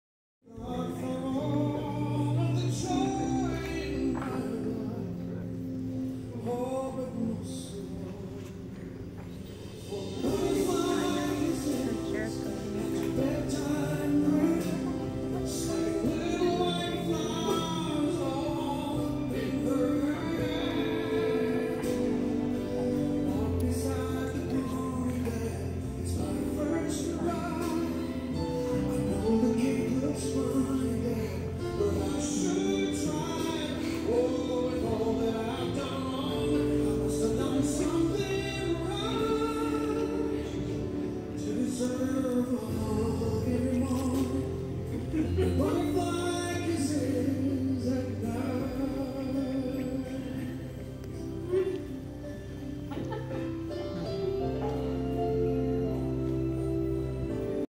Father of the Bride Dance
Dad dances with his daughter.